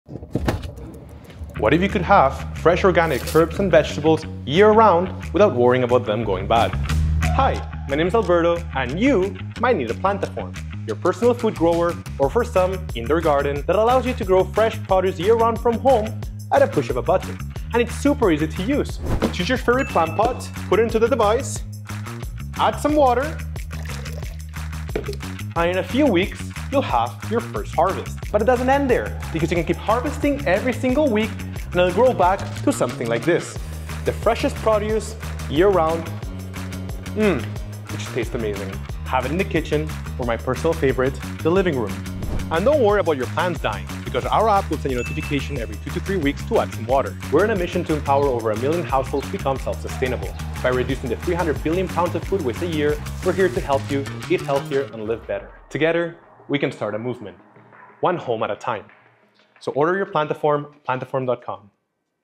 Plantaform Indoor Garden: Sustainable Living Starts Here
Take the first step towards sustainable living with the Plantaform Indoor Garden! 🌱

Designed to bring fresh, homegrown greens into any space, this innovative garden makes it easy to grow herbs, leafy greens, and more—all year long.

With advanced technology, sleek design, and an effortless growing experience, the Plantaform Indoor Garden helps you embrace a healthier, greener lifestyle. Whether you’re a beginner or an experienced gardener, it’s the perfect way to cultivate your favorite plants sustainably.

Join the growing movement for sustainable living today!

📱 Follow us for more inspiration and updates!